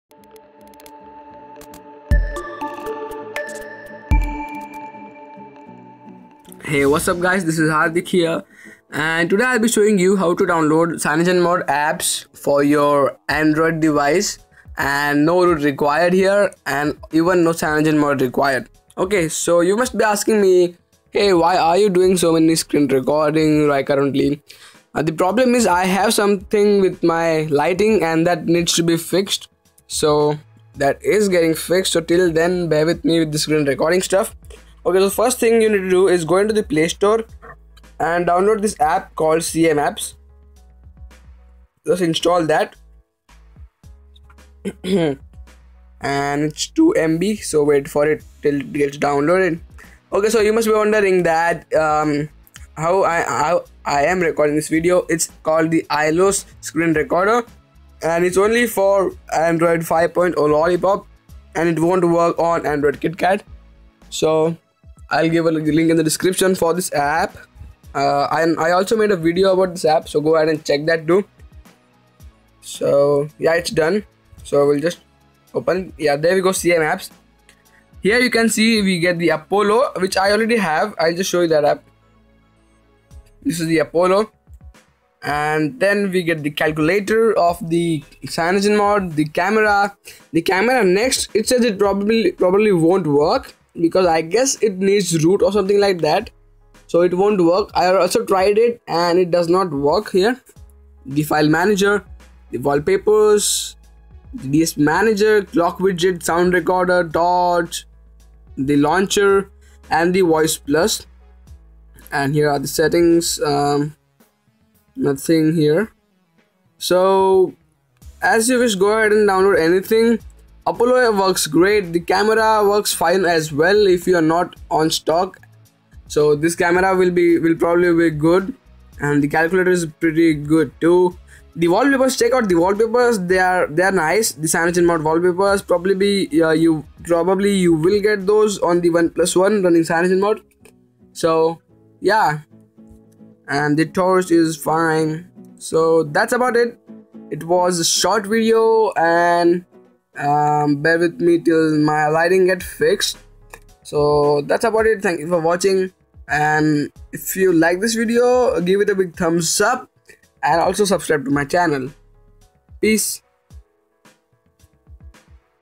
How To: Download CyanogenMod Apps Free(No Root)
In this video i will be showing you how to download Cyanogenmod Apps free on your Android Device!